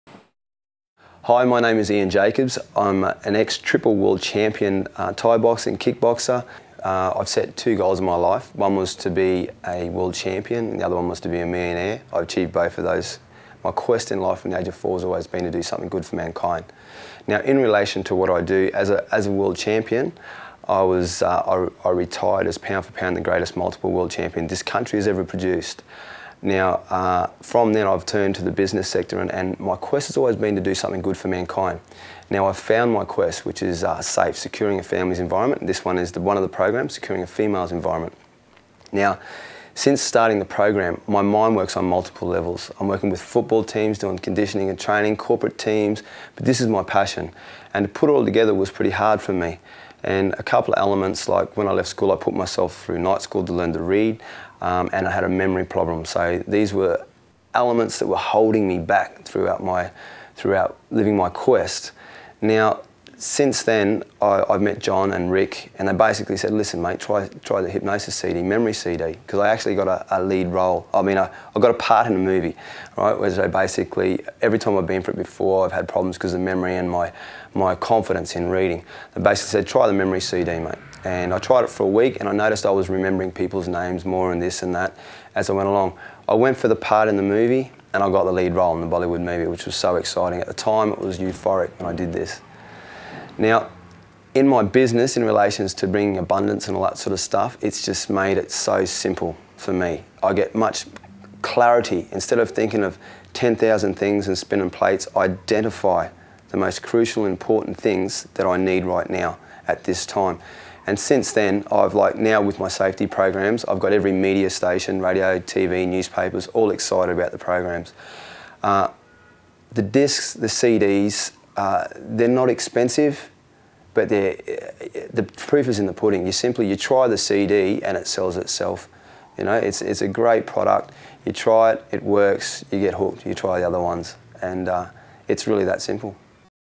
Hypnosis & Ian Jacobs
Ian Jacobs, triple world champion kickboxer and Muay Thai fighter, discusses how Rick Collingwood self hypnosis Cds and MP3s helped him improve his memory and achieve a role ina  bollywood movie. This is a testimonial for Mind Motivations and Resonanz Recordings range of rick Collingwood Life Improvement Hypnosis Cds and Mp3s.